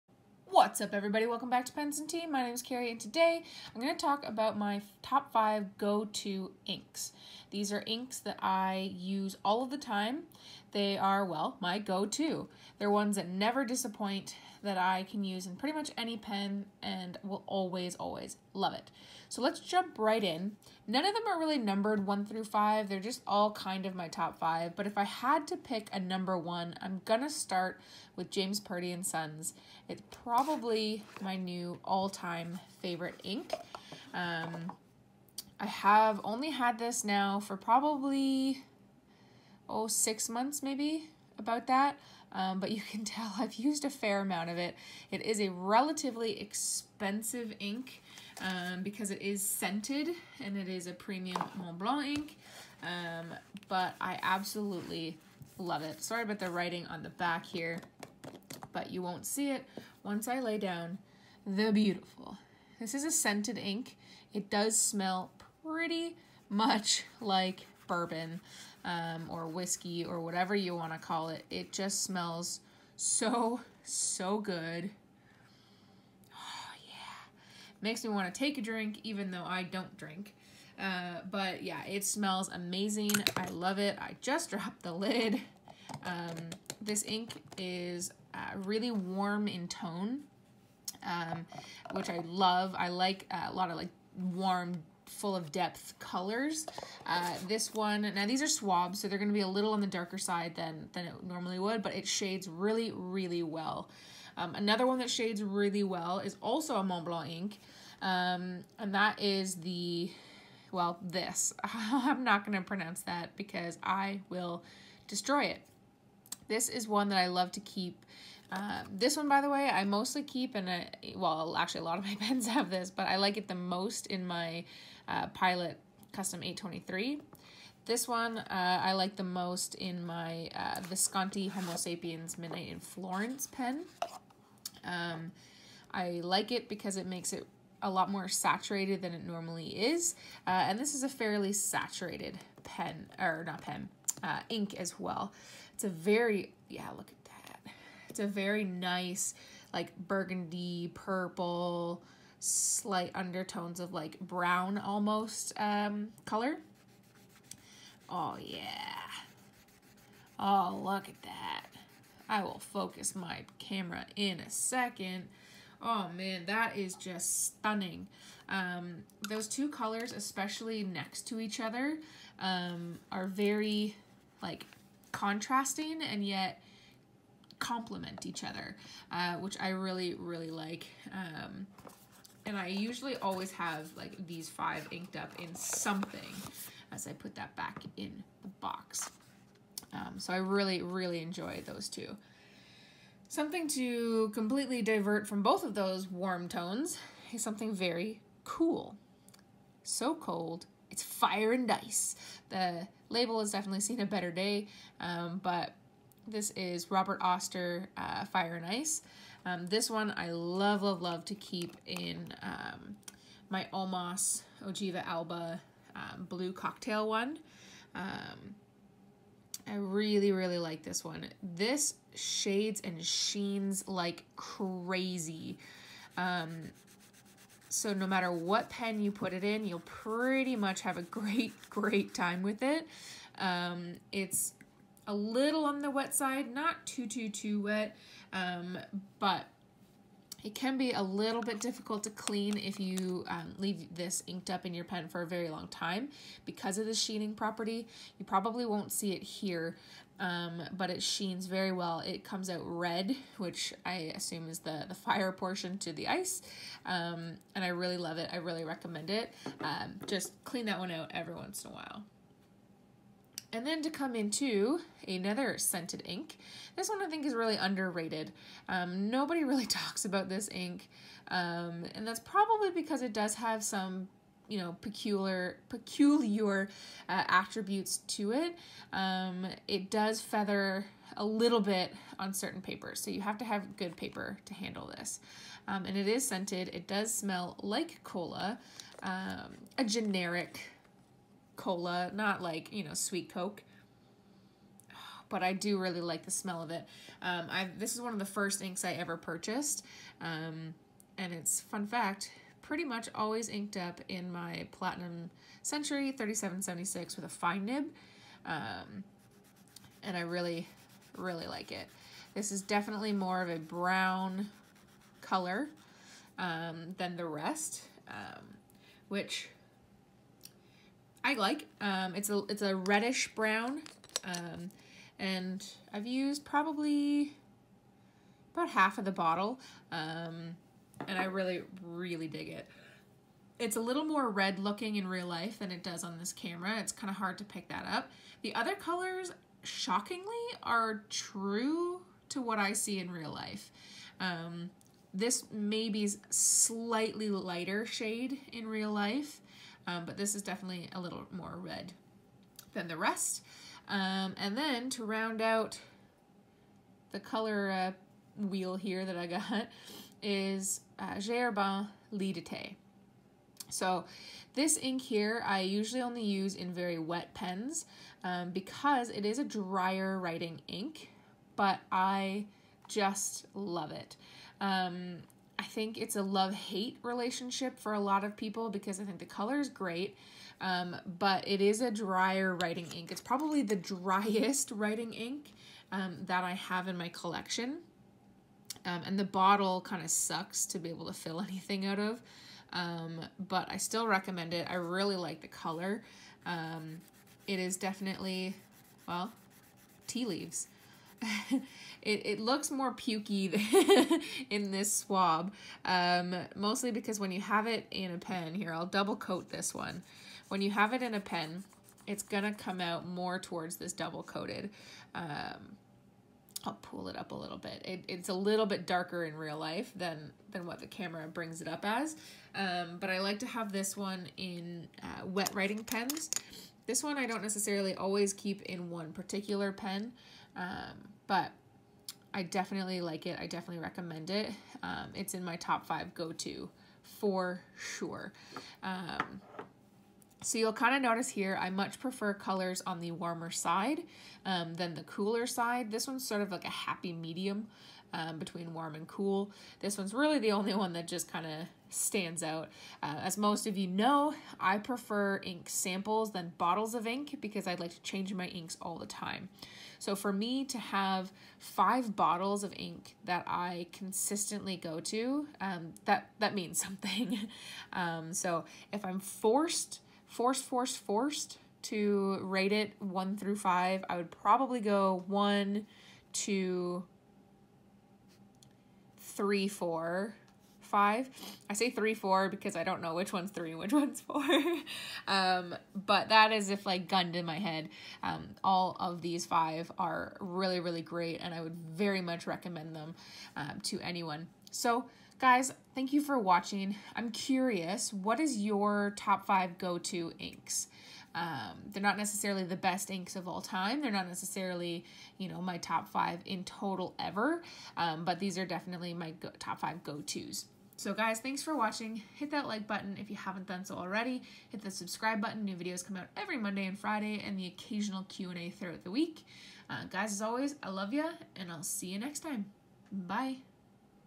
Top 5 "Go To" fountain pen inks
I talk about the inks that I find myself "going to" all the time!